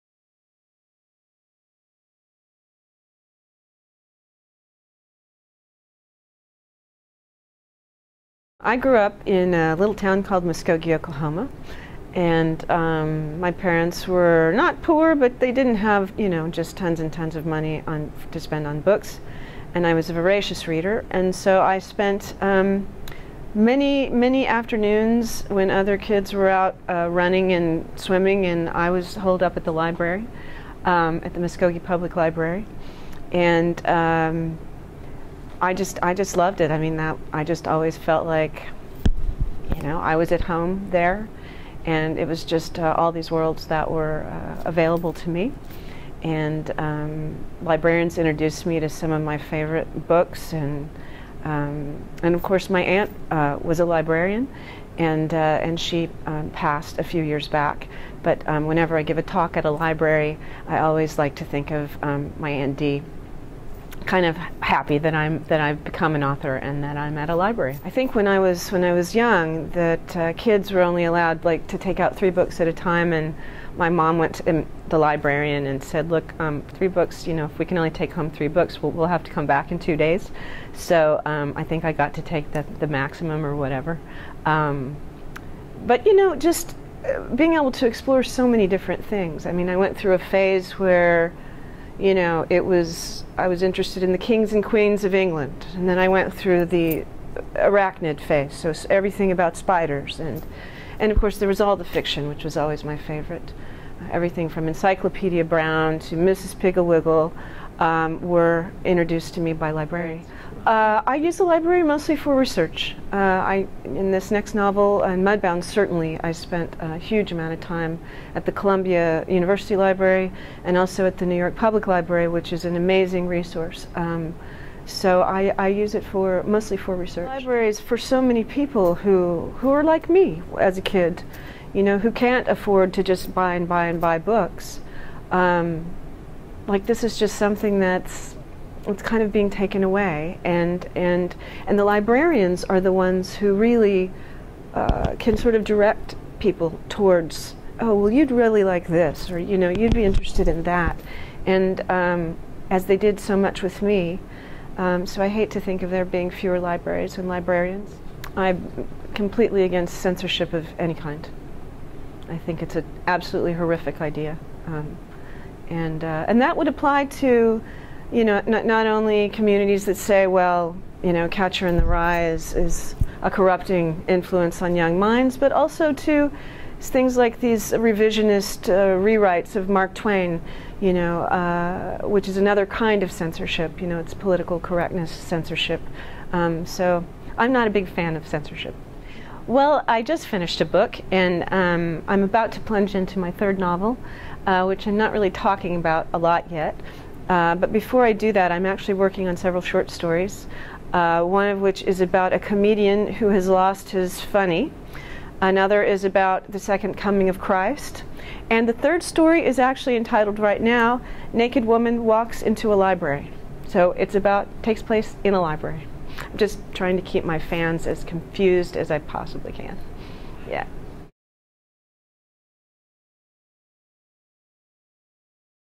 Hillary Jordan: 'Librarians introduced me to some of my favorite books'
Hillary Jordan, author of "When She Woke" shares her library stories.